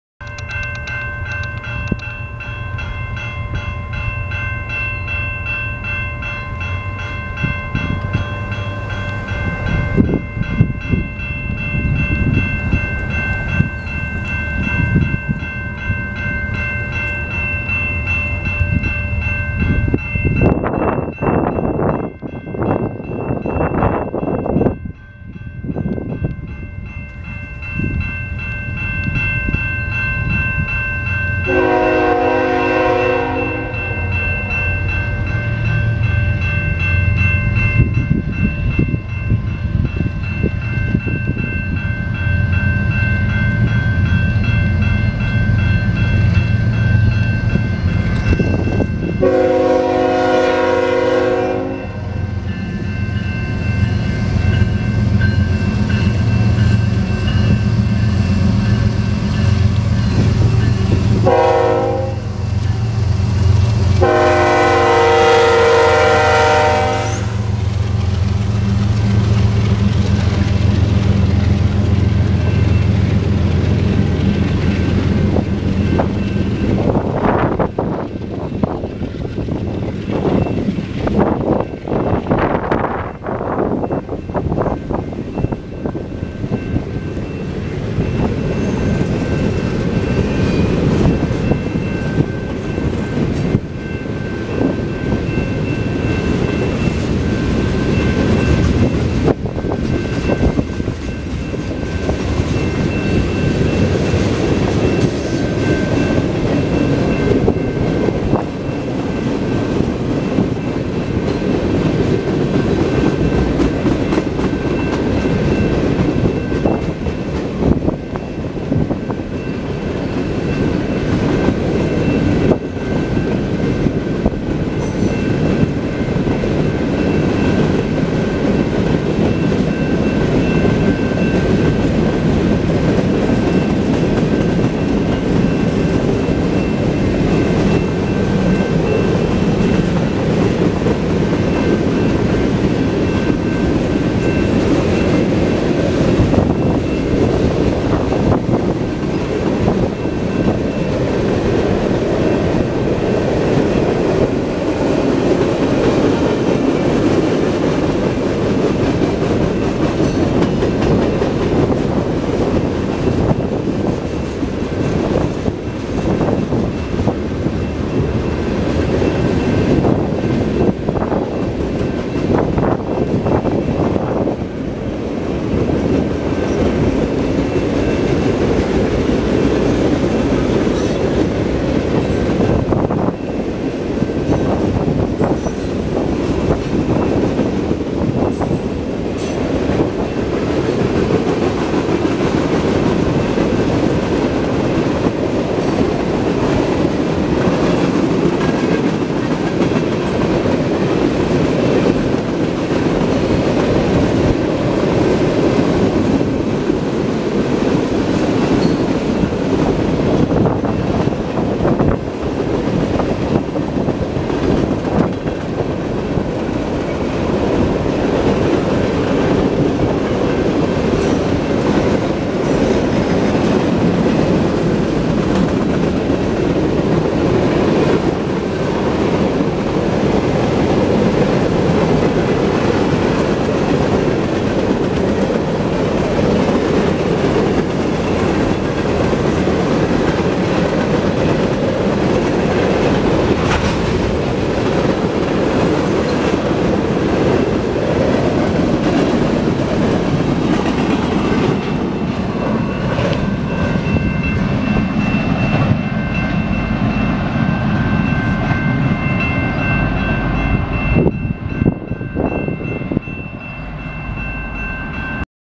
Canadian National Tier 4 Port Huron MI (5-3-2019)
Lead engine: CN Sticker Emblem #3209 GE ET44AC
Helper engine: CN #2862 GE ES44AC Built 11/2013